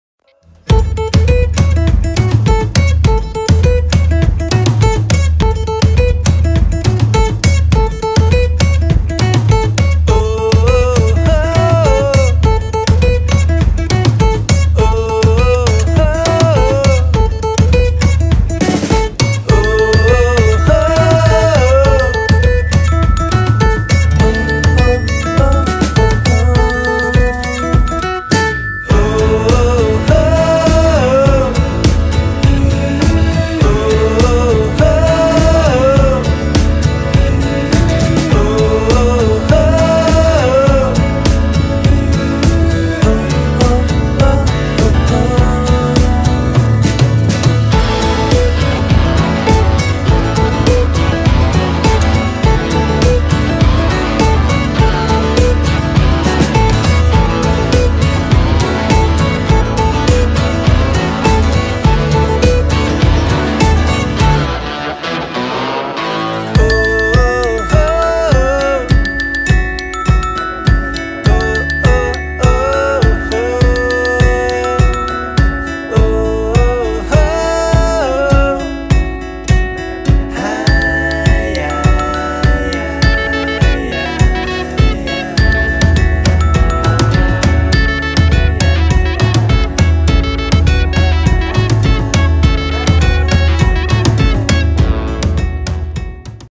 Clarinbridge, Lisduff/Caherbulligin Turlough (Winter Lake)
River bursting its banks!